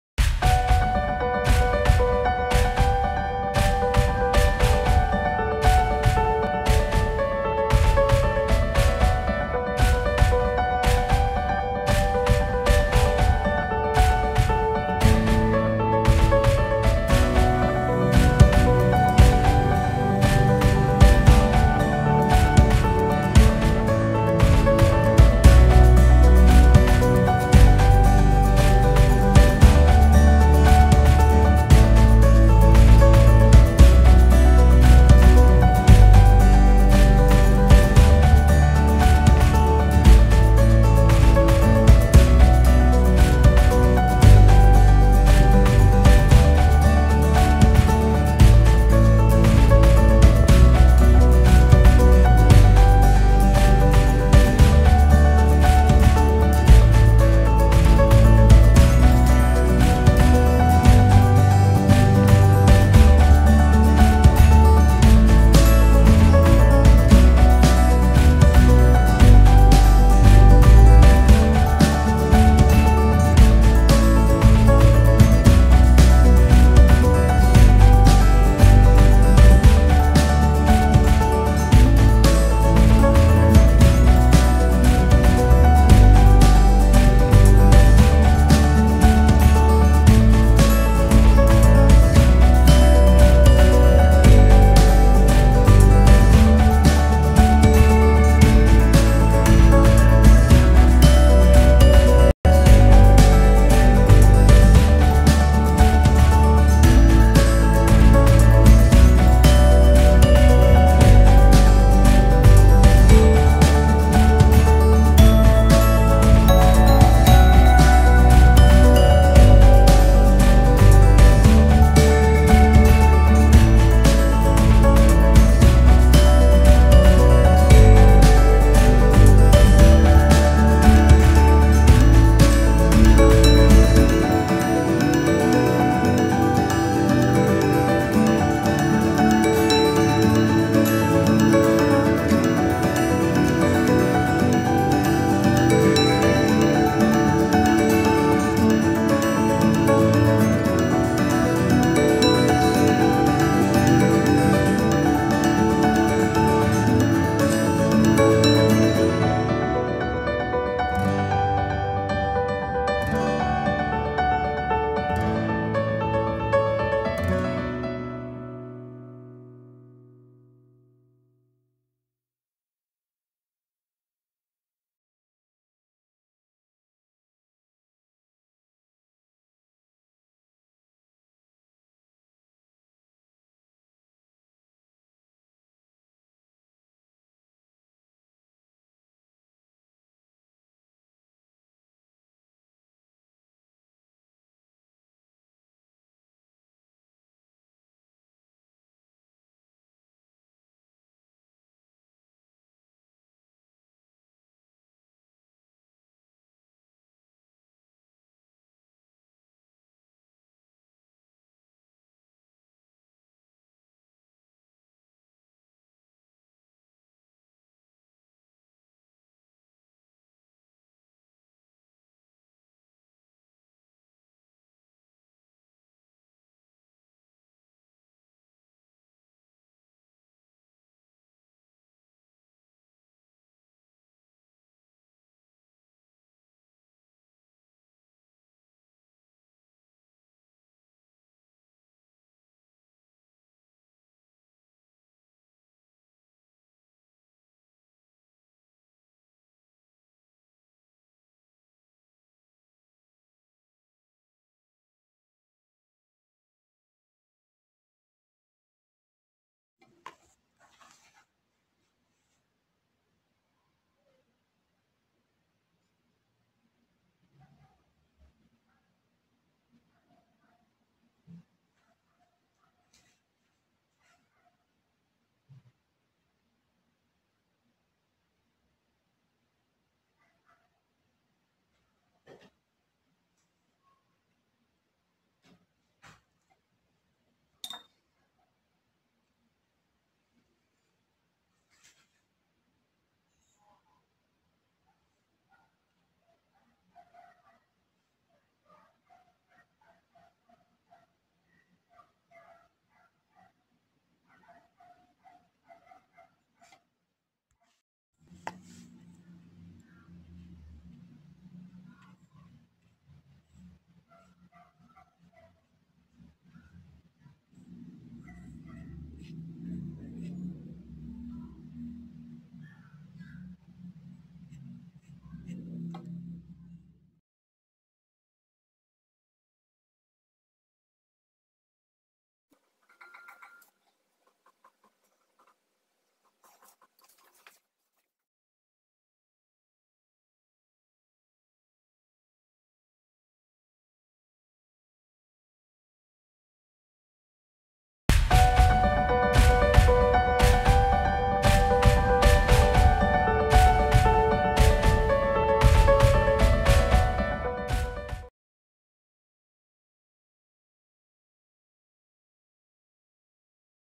Botanical watercolor lessons at Hanbury Gardens online video tutorial - Eighth lesson: AGAVE WEEVIL
Eighth video lesson of botanical painting at the Hanbury Botanical Gardens dedicated to Agave weevil (Scyphophorus acupunctatus)  with the botanical painter Chiara Trinchieri.